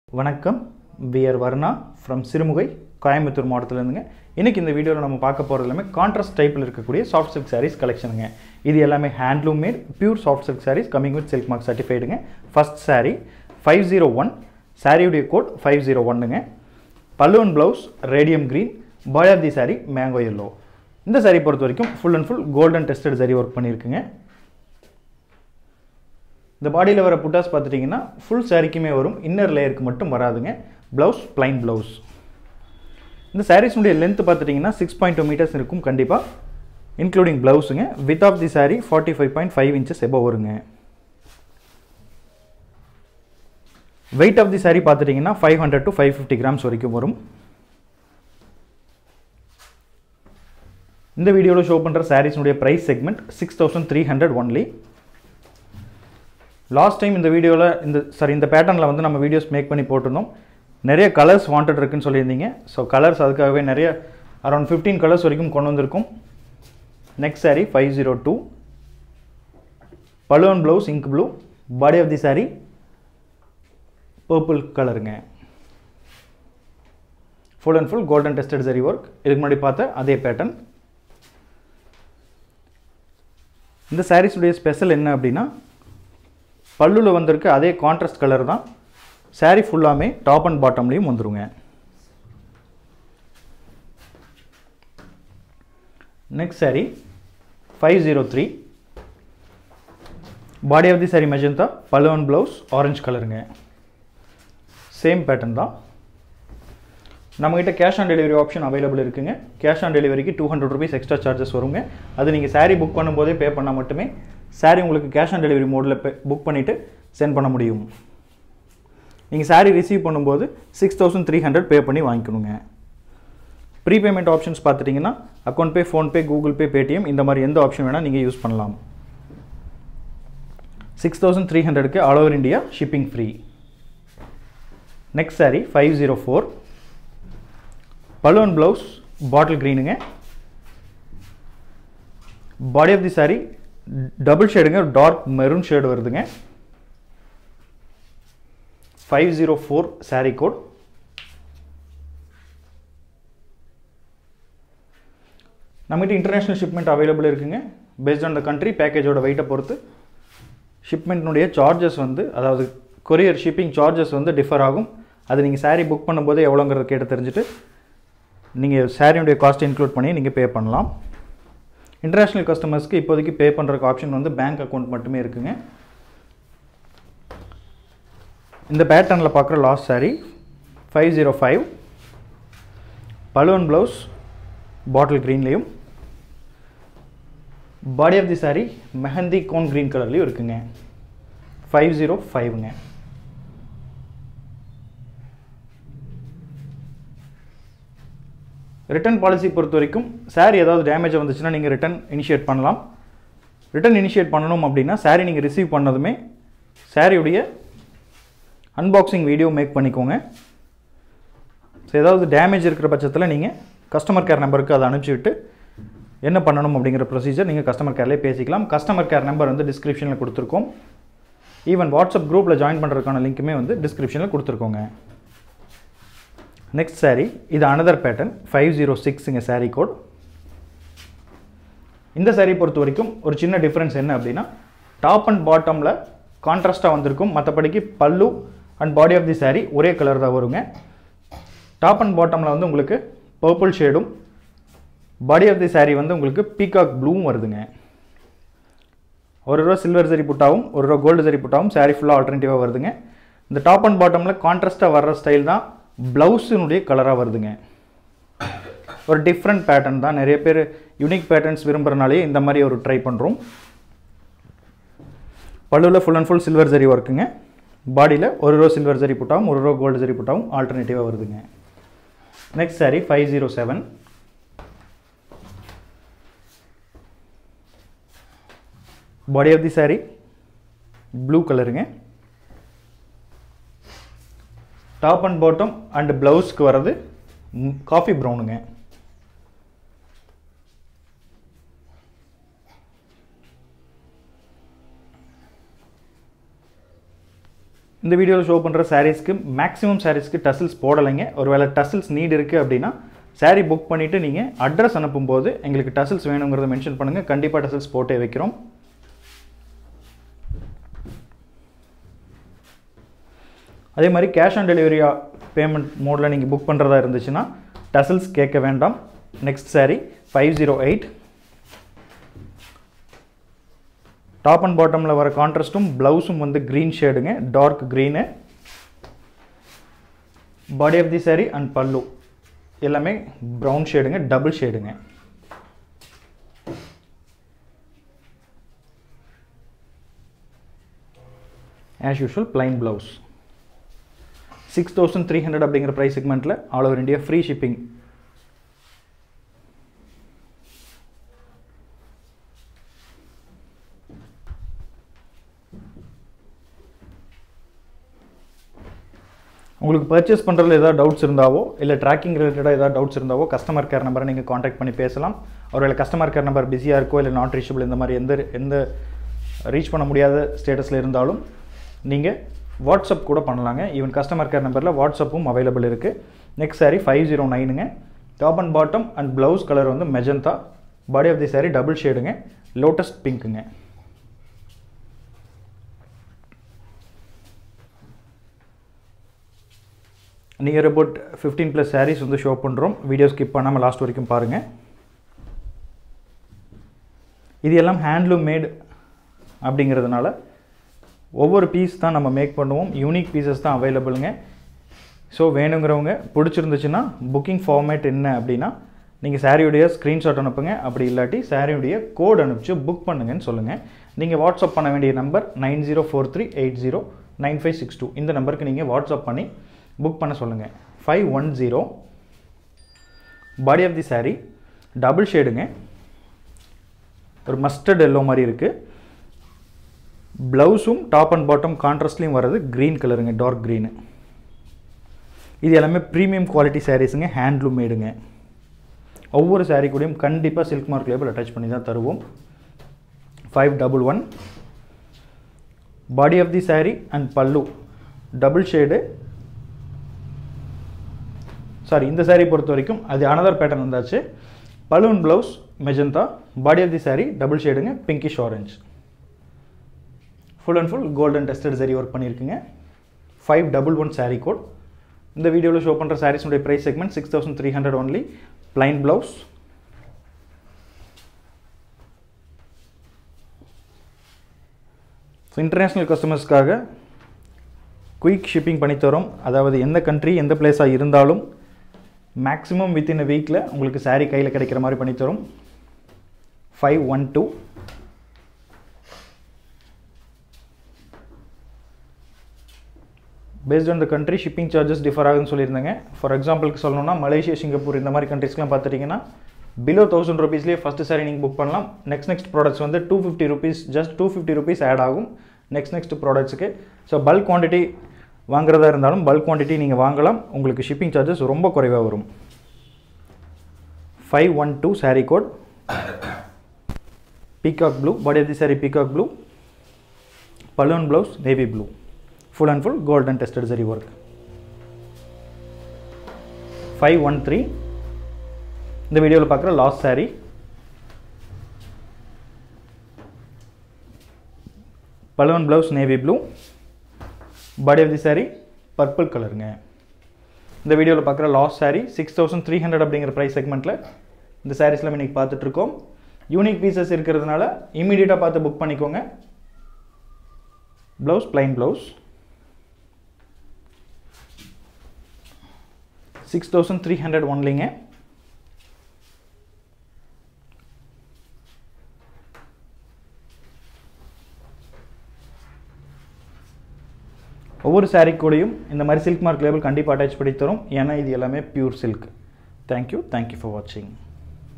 Contrast Pattern Soft Silk Sarees With Price | Latest Silk Sarees Design | Varnaa | 22/02/22
Contrast Pattern Soft Silk Sarees @ 6300/-

Handloom made | Pure Soft Silk Sarees | Double Warp |  Light Weight | with Silk mark | Cash on delivery | International shipping

Varnaa
11/137c, cheran nagar,
Annur Road
Sirumugai ,
Coimbatore dt.
Tamil nadu